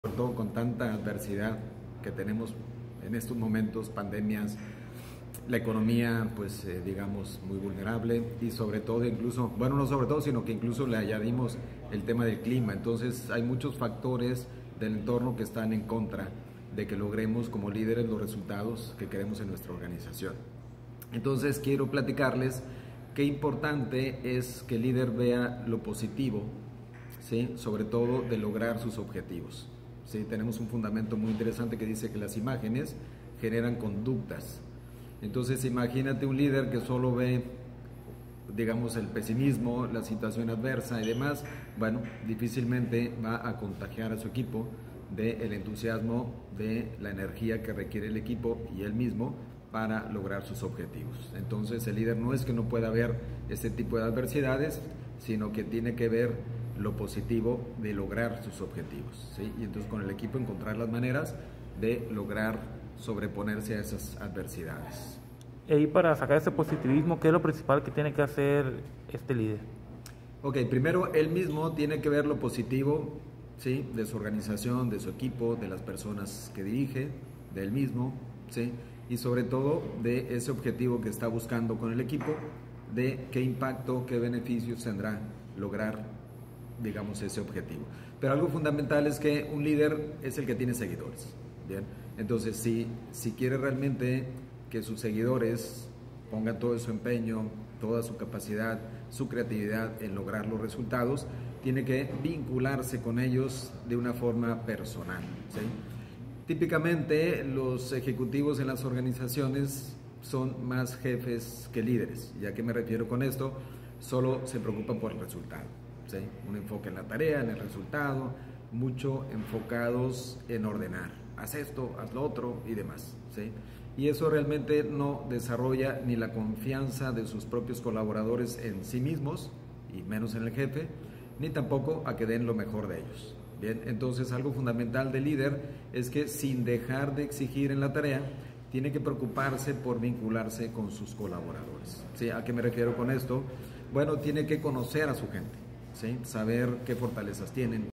Difícil que una empresa perdure, sin un liderazgo positivo
Guamúchil, Sin.- Es esencial que las empresas tengan un liderazgo positivo, si es que desean prosperar y mantenerse activas, así lo expresó el consultor Giancarlo Von Nacher, quien señaló que debido a las adversidades que se tienen actualmente como la pandemia, la mala economía e incluso los malos climas, son factores que se ponen en contra para que se logren los resultados de las organizaciones.